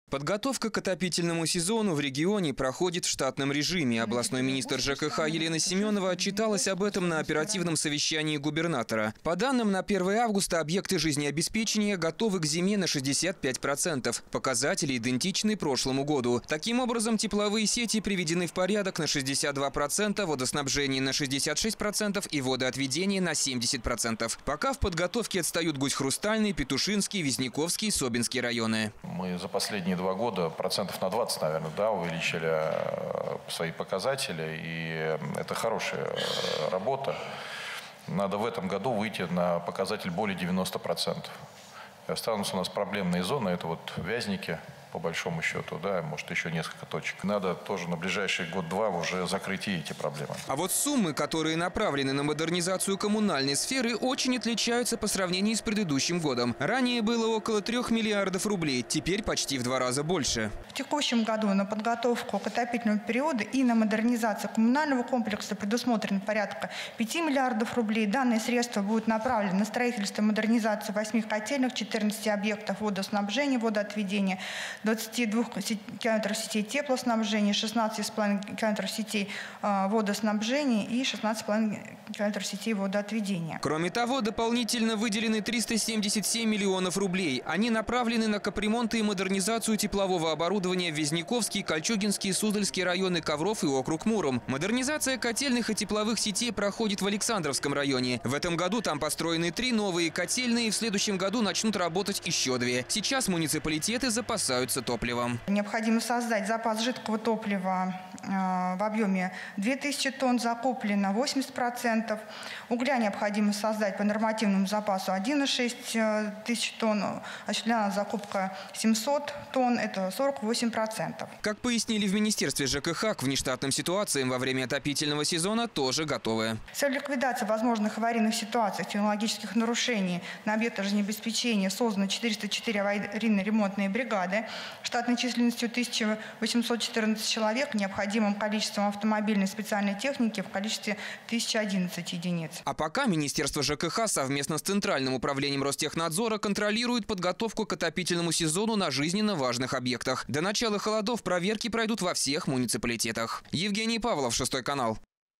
Владимирская область потратила 5 миллиардов на подготовку к отопительному сезону
Подготовка к отопительному сезону в регионе проходит в штатном режиме. Областной министр ЖКХ Елена Семенова отчиталась об этом на оперативном совещании губернатора. По данным на 1 августа, объекты жизнеобеспечения готовы к зиме на 65 процентов. Показатели идентичны прошлому году. Таким образом, тепловые сети приведены в порядок на 62%, водоснабжение на 66% и водоотведение на 70%. Пока в подготовке отстают Гусь-Хрустальный, Петушинский, Вязниковский и Собинский районы.